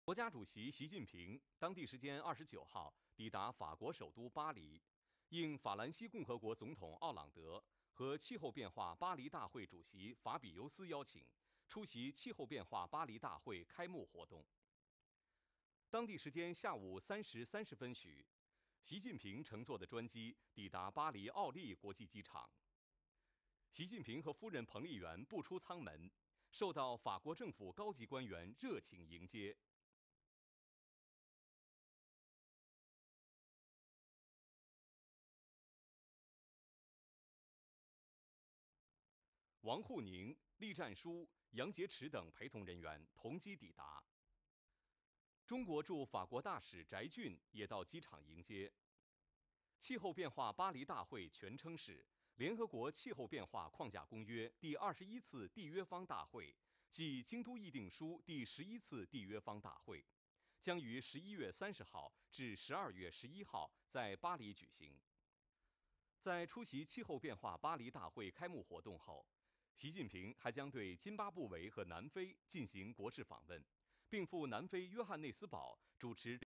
Chinese President Xi Jinping Arrives in Paris for COP21 Conference
Chinese President Xi Jinping arrived in Paris on Sunday to attend the opening ceremony of the 21st UN conference on climate change at the invitation of French President Francois Hollande and conference chairman Laurent Fabius.

This year's event, held in Paris from November 30 to December 11, is the 21st annual session of the Conference of the Parties (COP21) since the 1992 United Nations Framework Convention on Climate Change (UNFCCC) and the 11th session of the Meeting of the Parties following the 1997 Kyoto Protocol.

Following his visit to the COP21, the president will pay a state visit to Zimbabwe on December 1 at the invitation of his Zimbabwean counterpart Robert Mugabe.

Xi will then make a three-day state visit to South Africa from December 2 at the invitation of South African President Jacob Zuma, where the Chinese president will chair the summit of the Forum on China-Africa Cooperation in Johannesburg.